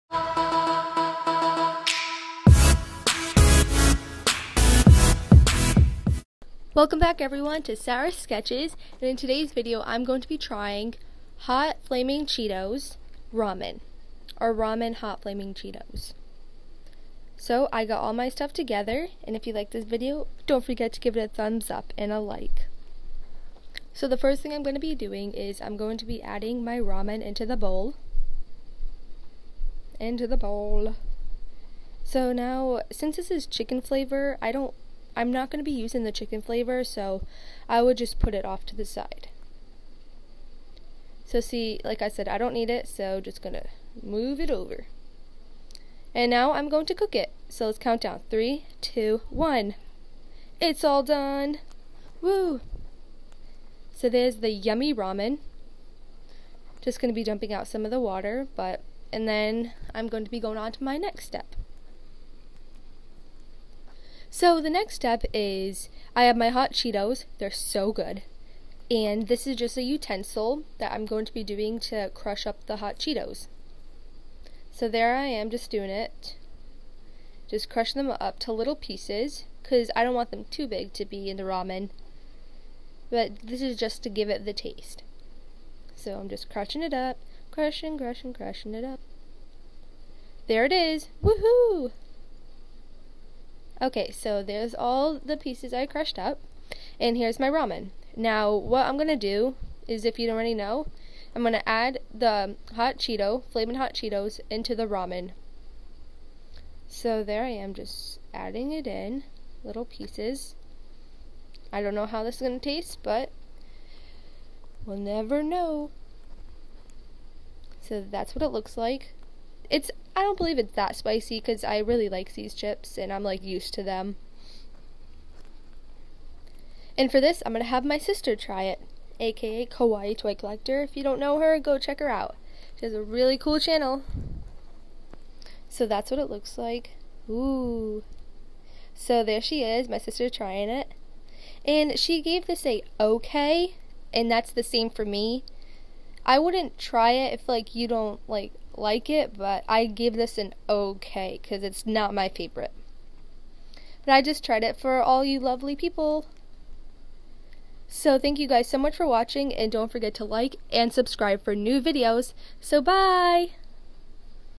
Flamin’ Hot Cheetos Ramen!!
Hello everyone, welcome back to another awesome video!

In today’s video, I will be attempting to make Flamin’ Hot Cheetos Ramen!

This is kinda a recipe of how to make it, it is fun and very easy to make!
~Things you need to make it: Ramen, Flamin’ Hot Cheetos, A plastic bag, utensil, and a Bowl.